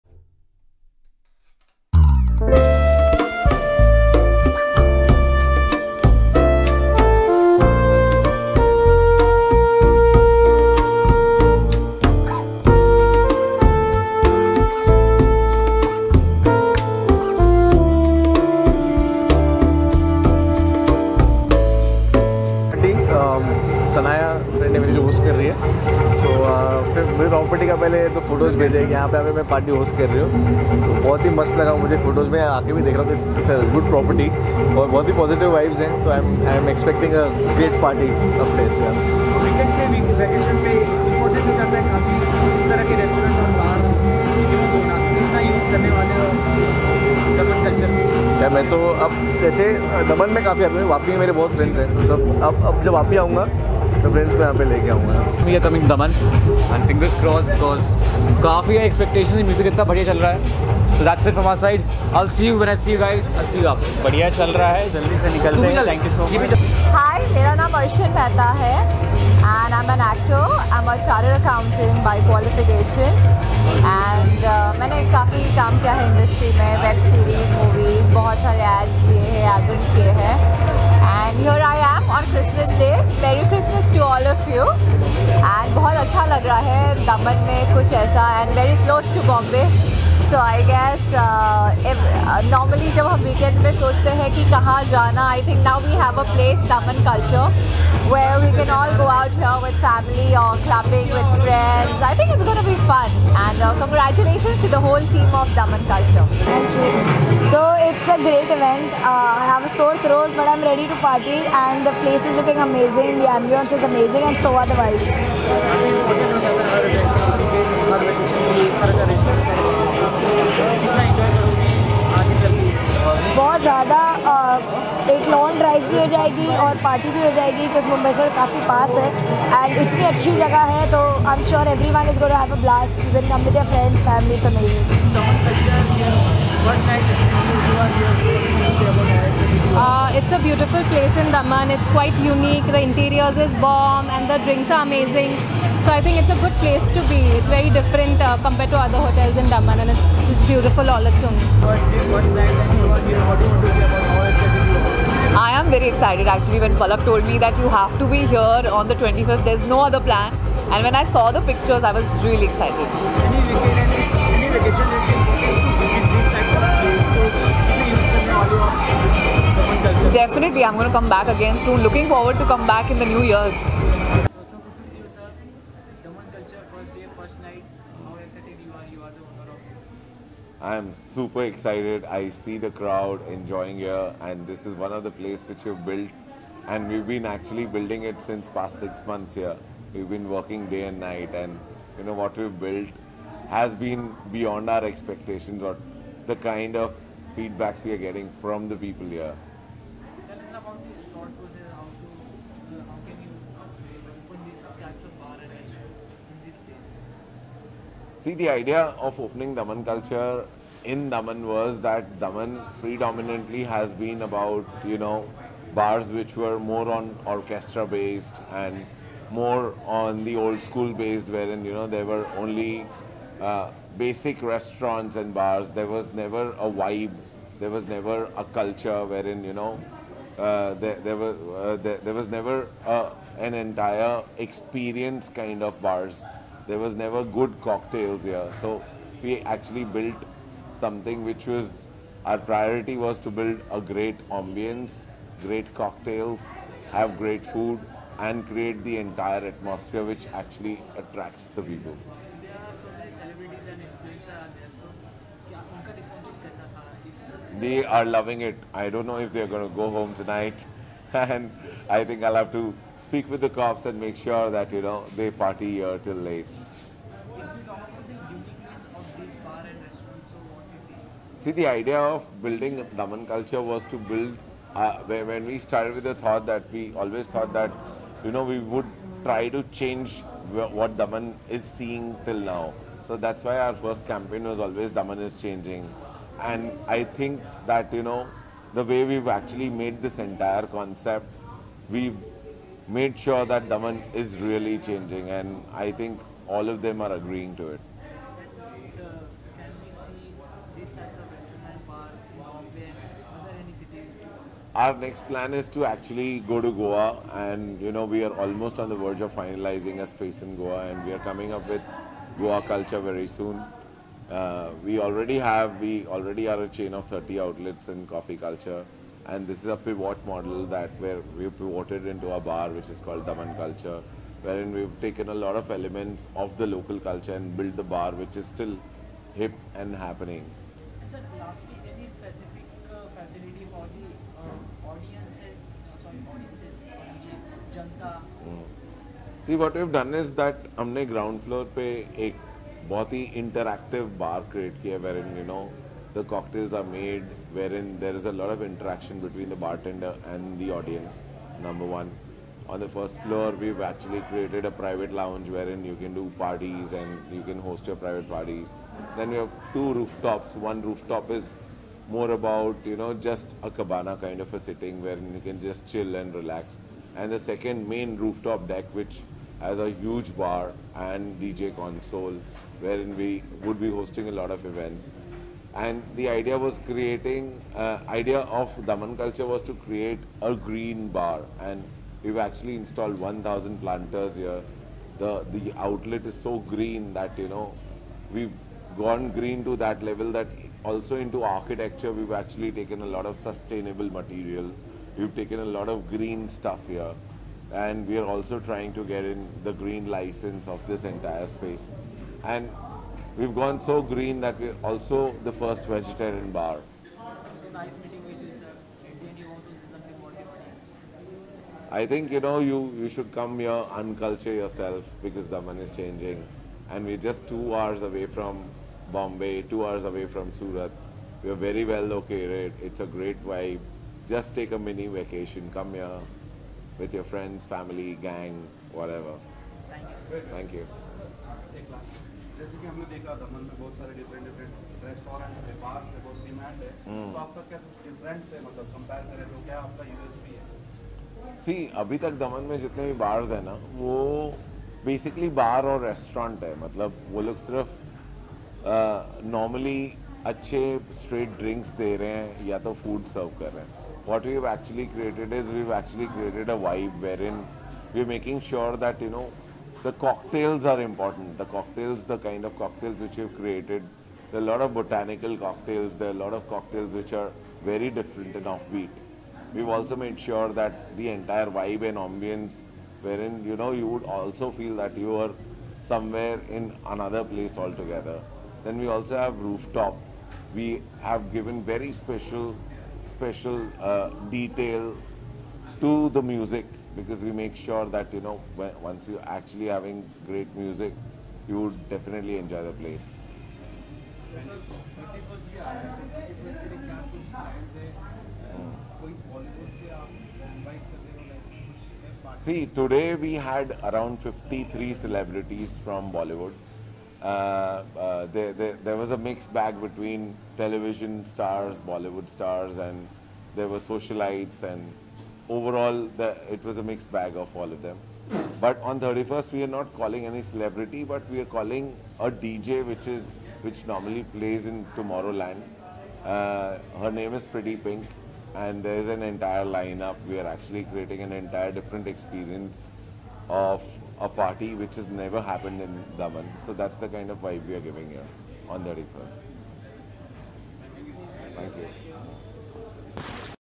Daman Culture - Sundowner Bar Launch | Christmas Party 2022 | Gaurav Narang
If you like India's best Hindi Blogs and Review Blogs in Hindi

Label - Wow Aparna